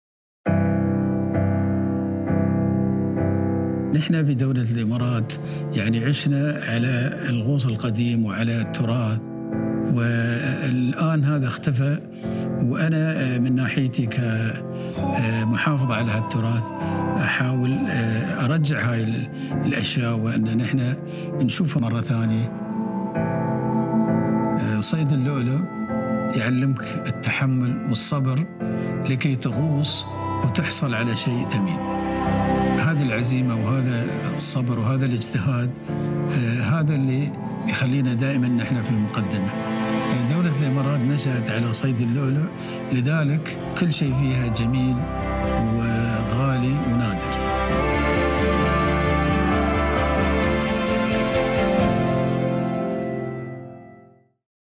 Preserving the tradition of pearl diving: Major Ali Saqr Alsuweidi
Major Ali Saqr Alsuweidi is the President of Emirates Marine Environmental Group and has played a prominent role in safeguarding the nation’s maritime heritage. Discover what drives him to preserve the tradition of pearl diving.
يشغل الرائد علي صقر منصب رئيس مجموعة الإمارات للبيئة البحرية، ويلعب دوراً بارزاً في حماية البيئة البحرية والمحافظة على إرث البلاد والغوص التراثي. اكتشفوا ما الذي يجعله "الحارس".